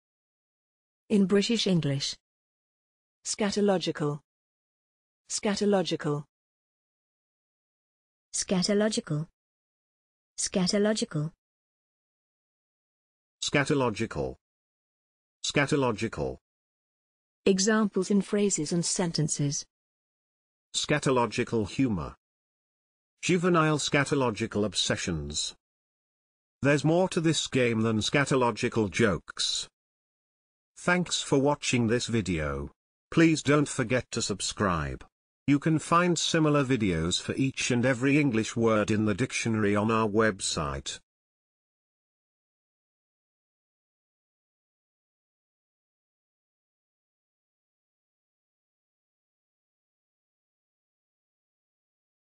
scatological - pronunciation + Examples in sentences and phrases
This is the pronunciation of scatological in British English + a few examples of its pronunciation in phrases and sentences.